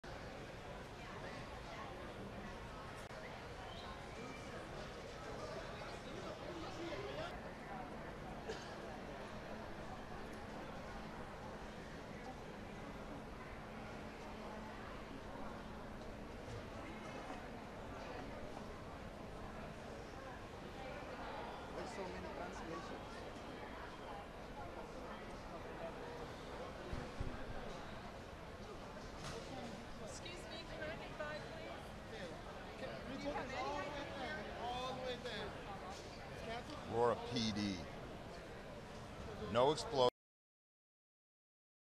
IL: FAA CENTER FIRE- O'HARE AIRPORT INTERIOR
FIRE AT AIR TRAFFIC CONTROL CENTER GROUNDS ALL FLIGHTS IN AND OUT OF CHICAGO.